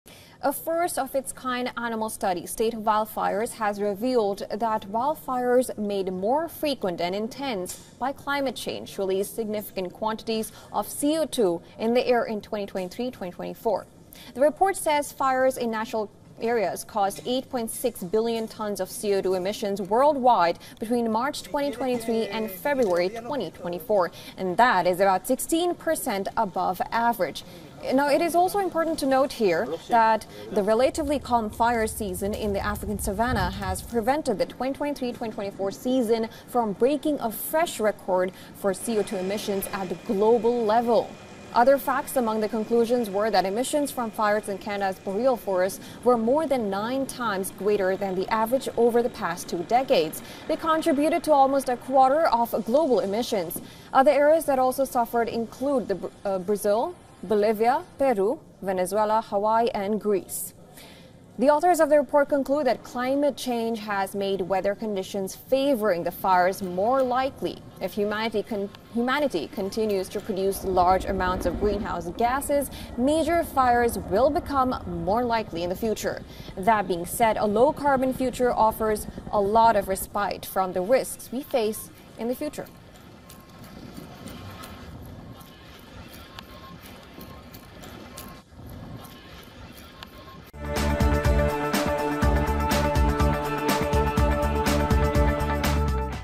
Wildfires raised 8.6 billion tonnes of CO2 emissions in 2023: Study | WION Climate Tracker
A first-of-its-kind annual study 'State of Wildfires' has revealed that wildfires made more frequent and intense by climate change released significant quantities of CO2 into the air in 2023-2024.